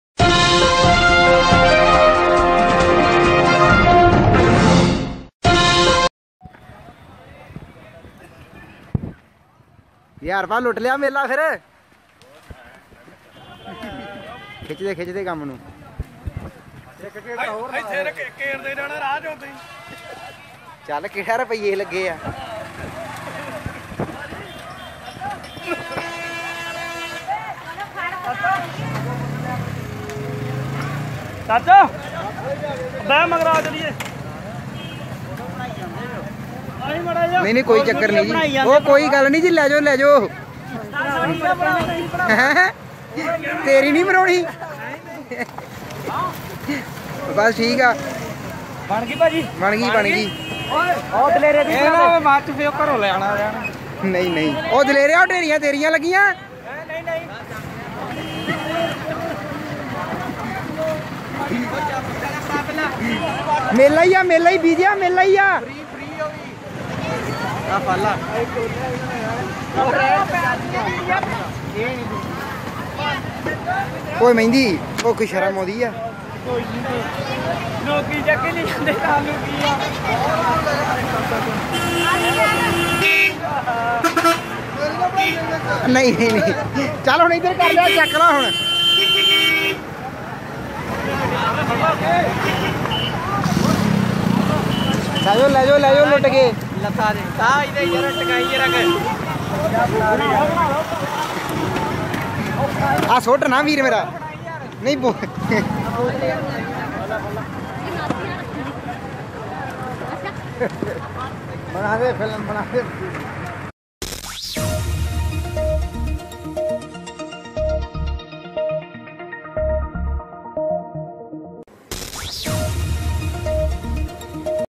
COCA-COLA BOTTLES LOOTED IN PUNJAB..HAWK MEDIA HOUSE
PUNJAB
A Truck carrying Coca-Cola bottles overturned and people started looting the bottles.
Youth women's and old age people were seen carrying the Coca-Cola bottles lying on the road.
HAWK MEDIA HOUSE